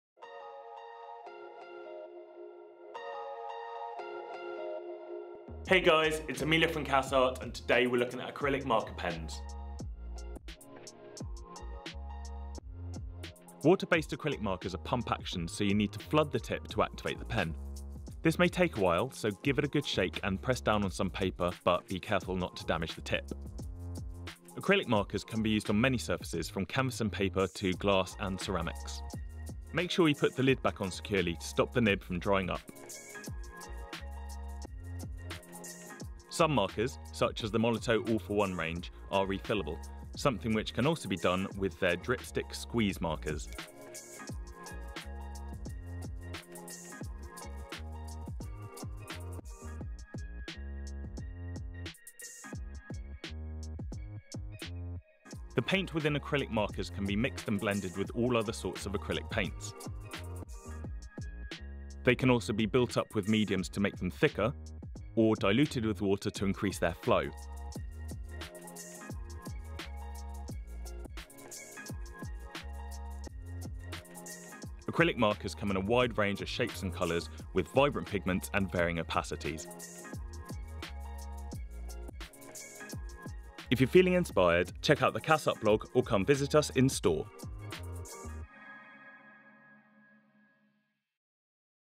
How to with Cass Art: Acrylic Markers
With broad Liquitex Acrylic markers, finers Molotov ONE4ALL pens and Golden high flow Acrylic watch Emilio build up his drawing into the bold and graphic bird that you see above - follow along to create your own graphic creation.